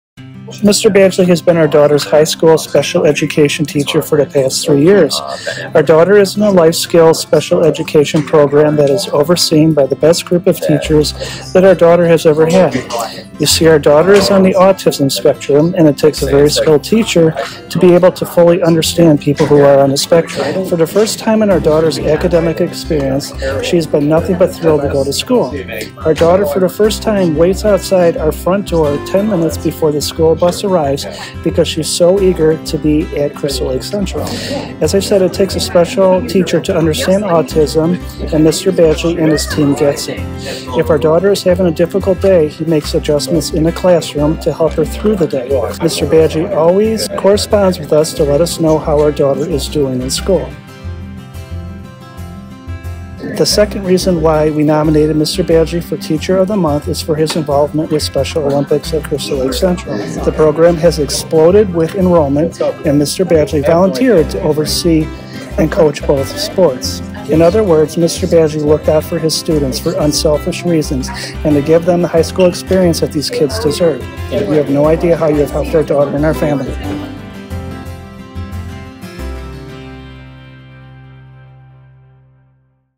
Crystal Lake Central's Dan Badgley Named Teacher of the Month
Crystal Lake Central's very own Dan Badgley was named Star 105.5 and Home State Bank's "Teacher of the Month"! Join us in congratulating Mr. Badgley! Take a look at how Mr. Badgley goes above and beyond every day for his students.